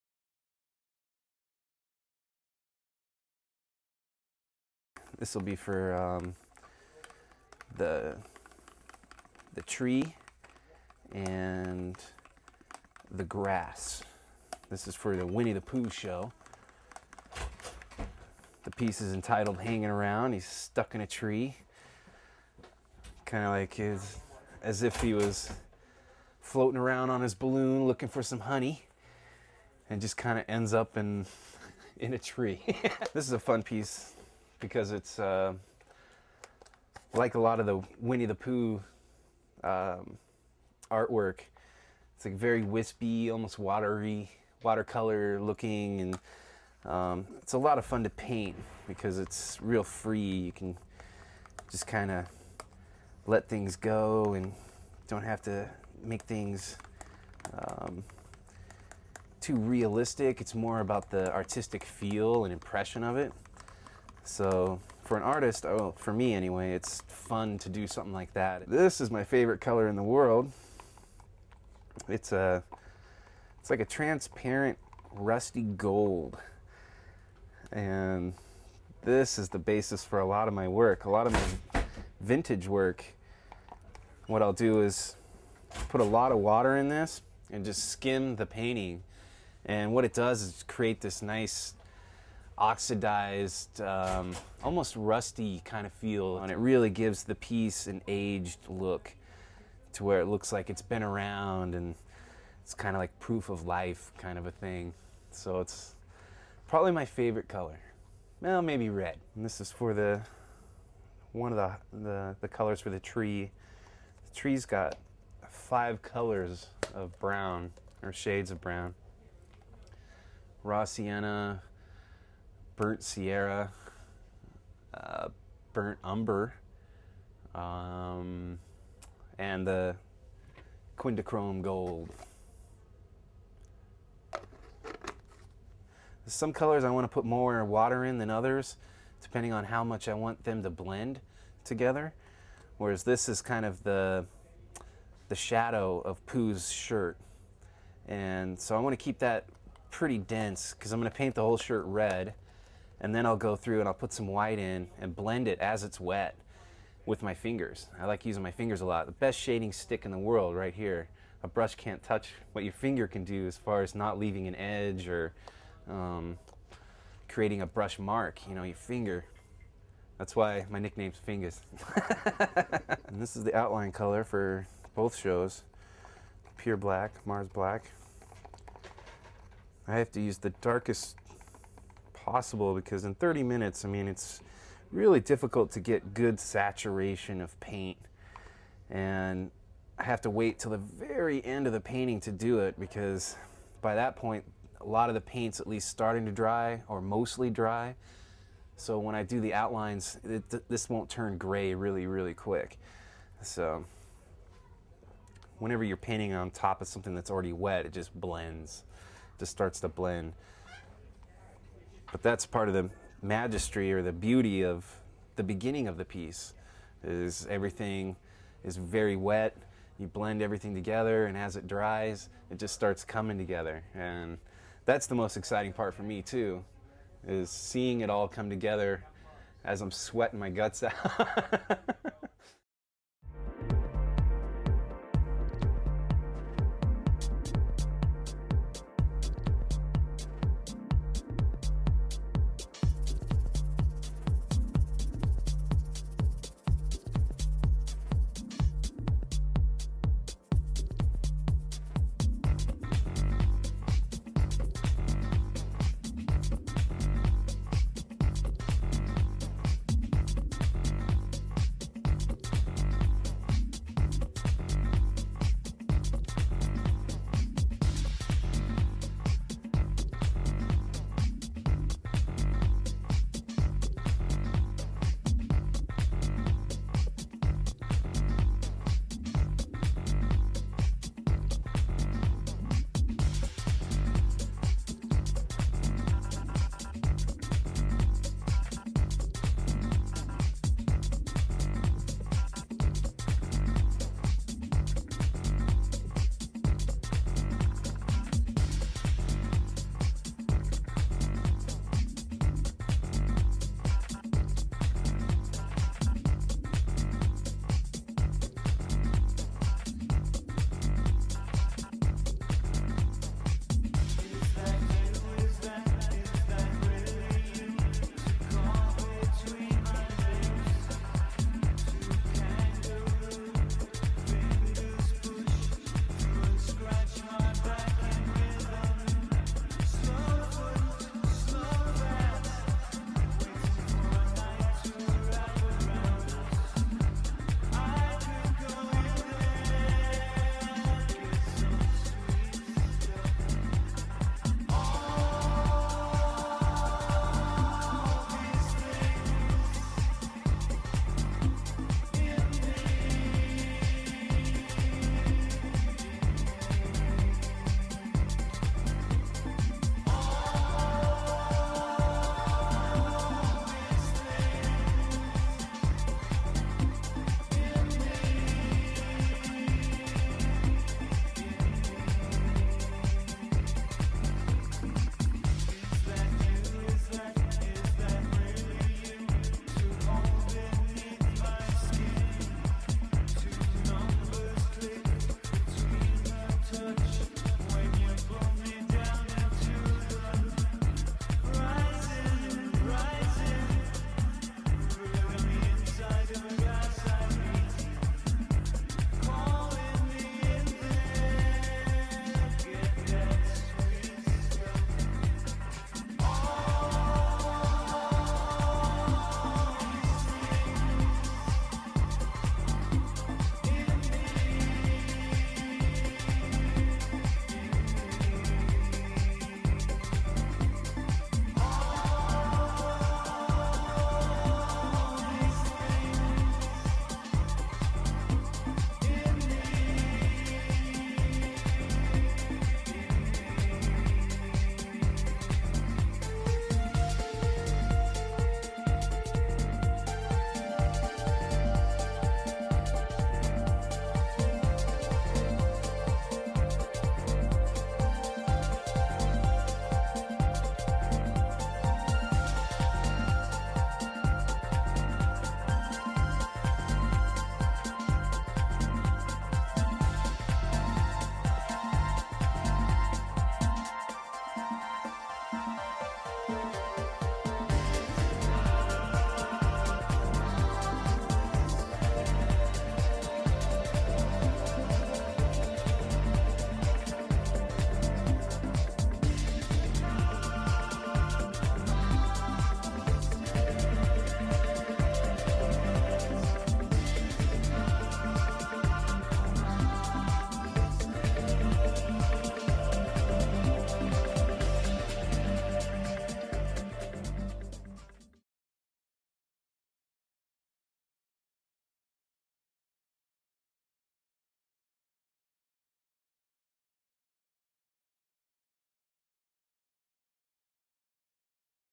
Pre-Show Interview with Trevor Carlton
Disney Fine Art's Trevor Carlton takes us through some of the preparation needed for his amazing performance pieces. We see Cartton in action as he paints disney characters like Tinkerbell and Winnie the Pooh.